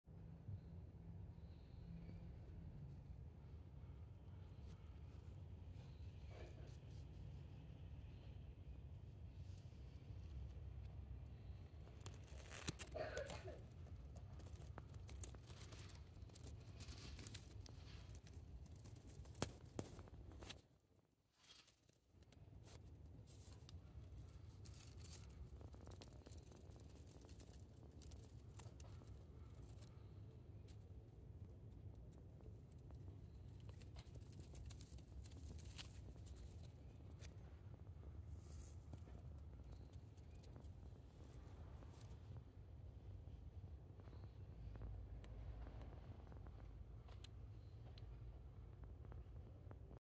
１０月のスーパーサルビア・ロックンロール
１０月のスーパーサルビア・ロックンロールの動画です。気温が下がってきましたが、まだまだ花が咲いています。